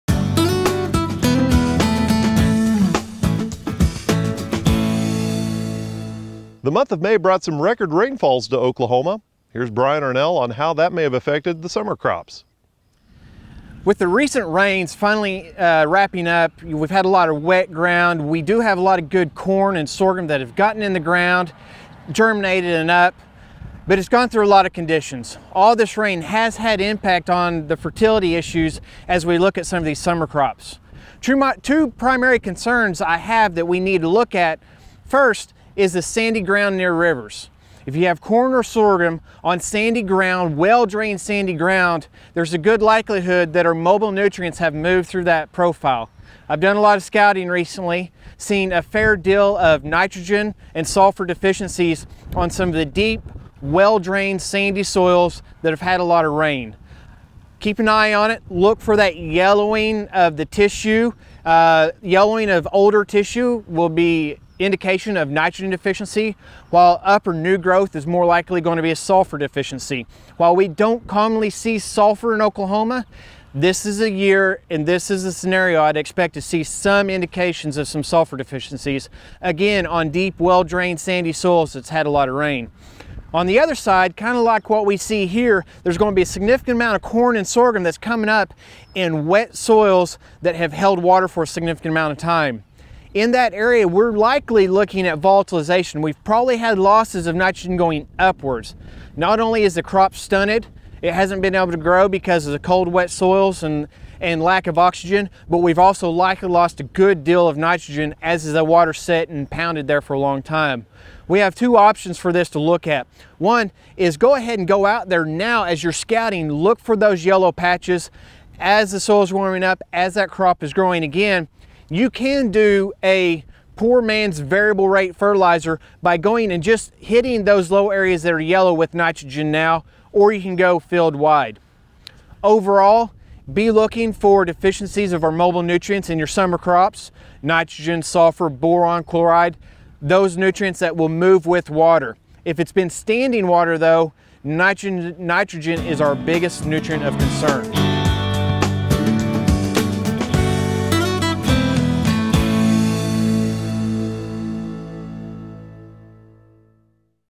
Nitrogen After Rain (6/13/15)
Brian Arnall discusses how Nitrogen can move through the soil following a heavy rain event. He also has recommendations for summer crop applications.